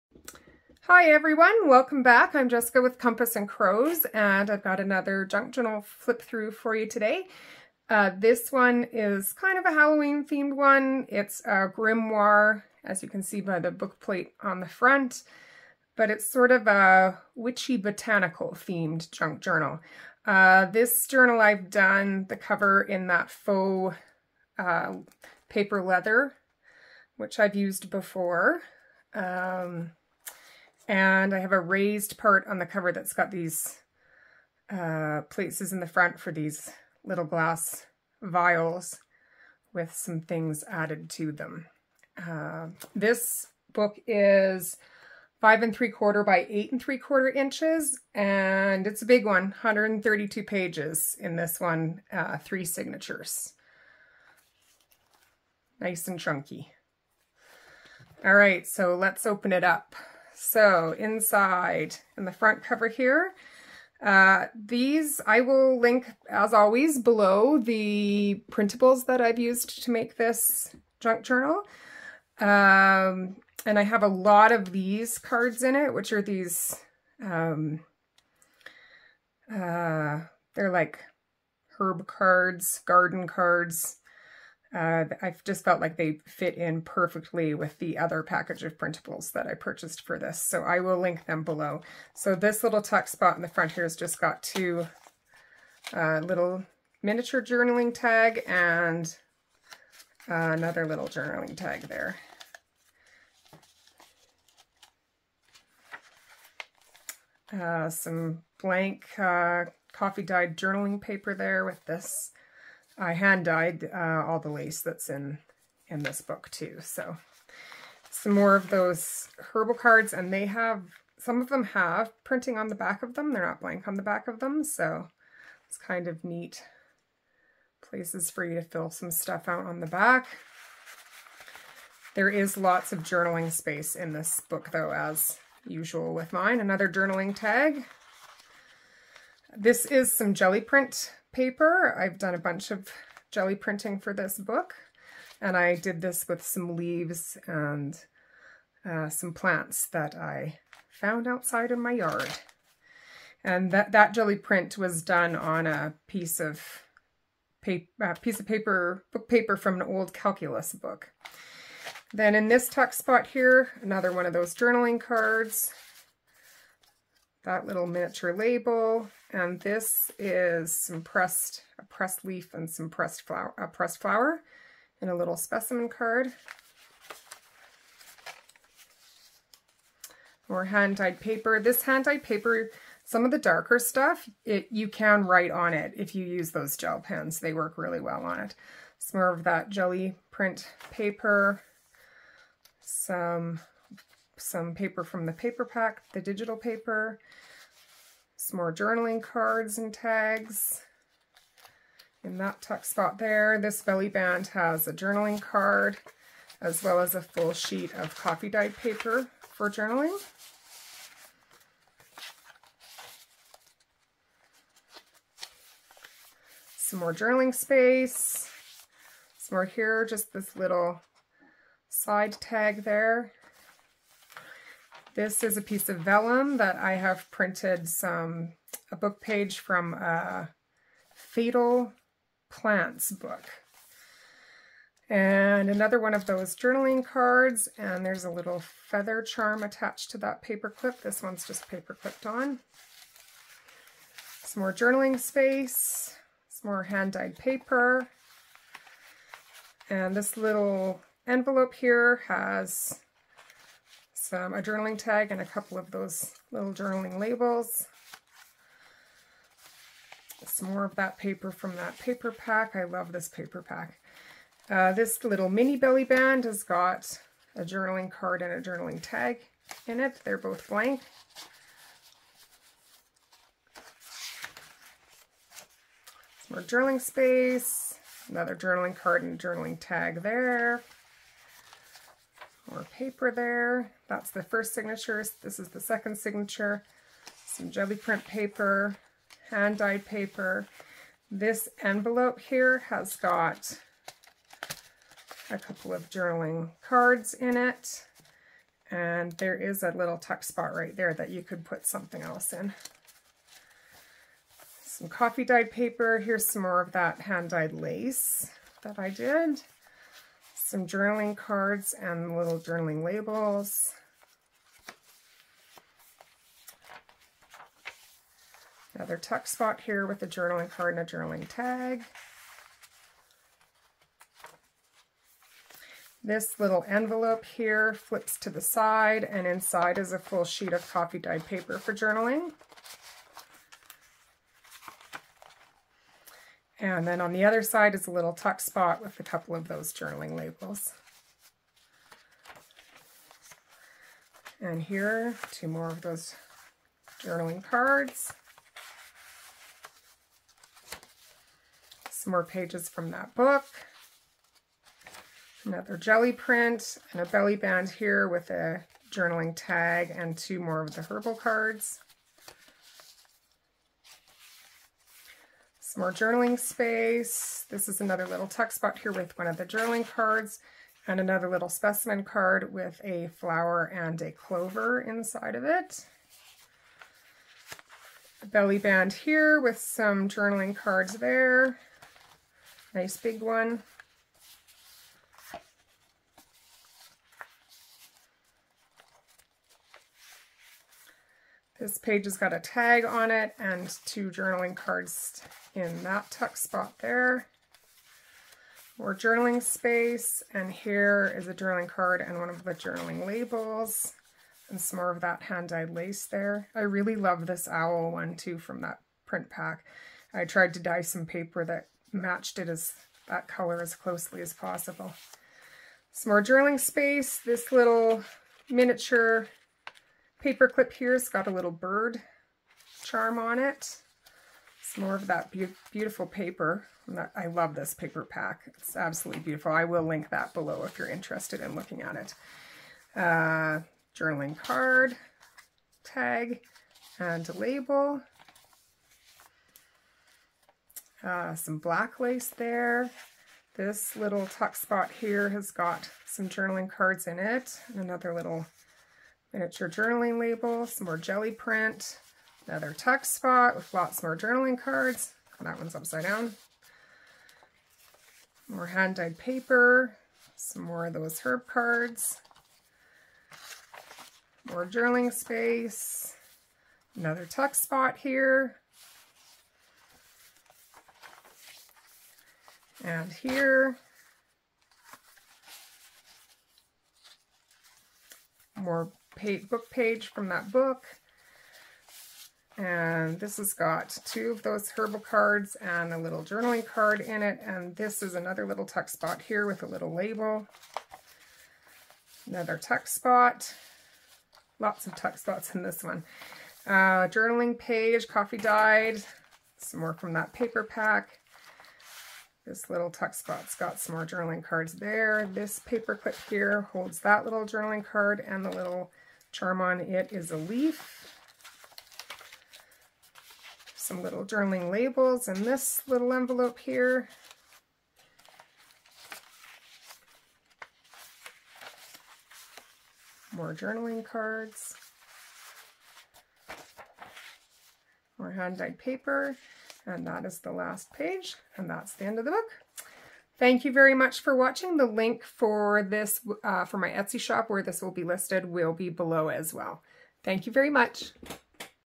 Witches Grimoire Junk Journal Flip Through (SOLD)
Paper kits used in this journal:
Healing Herbal Cards - SAACIBO
Cottage Witch Kit - GypsyCraftery